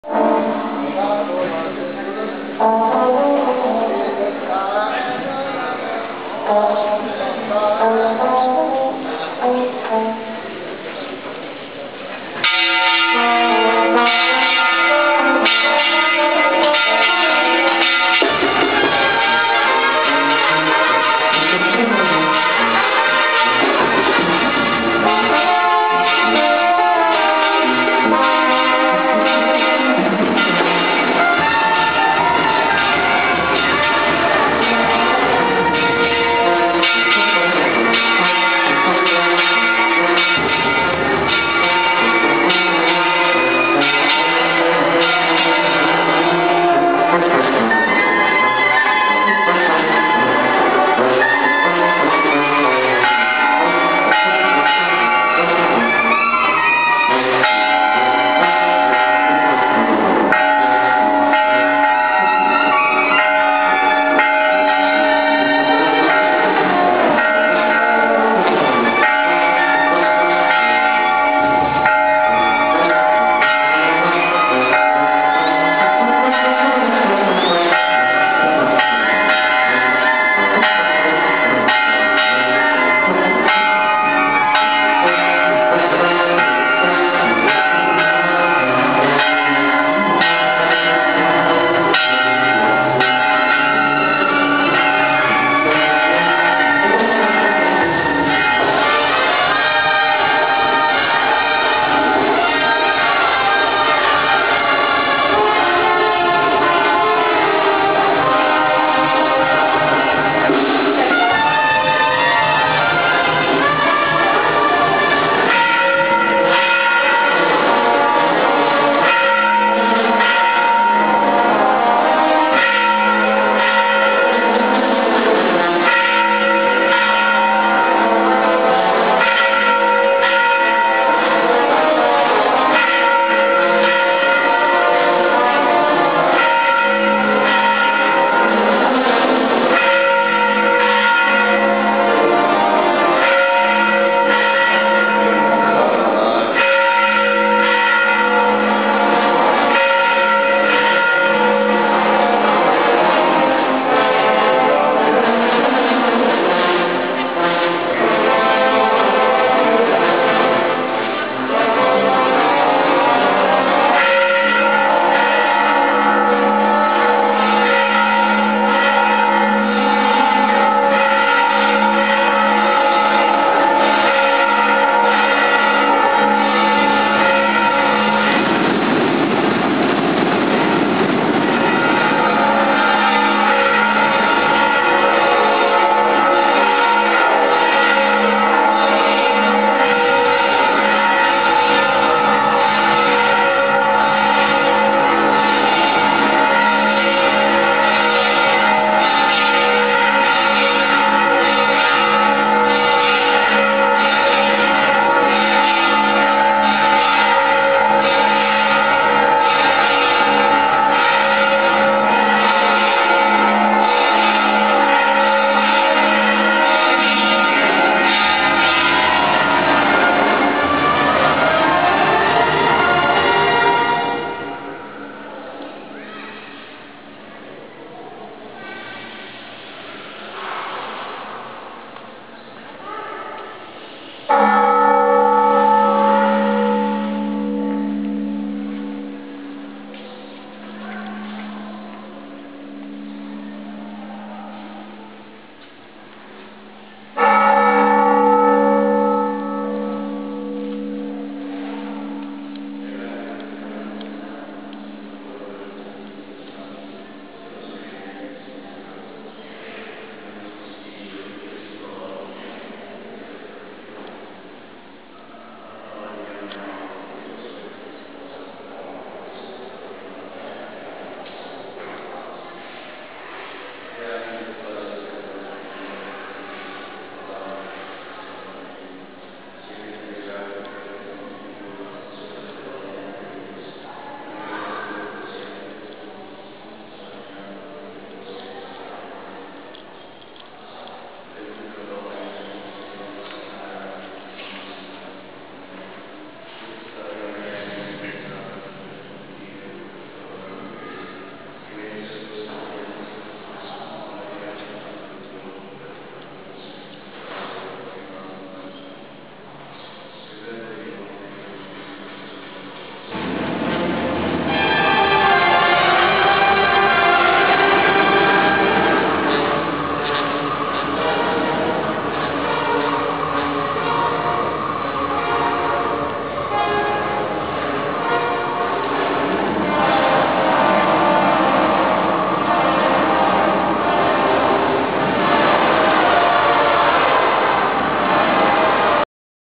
Teocuitatlán de Corona Jalisco 2009
Aspectos generales del Arrelgo del templo parroquial de Teocuitatlán de Corona con motivo de sus fiestas patronales en honor a la Inmaculada Concepción.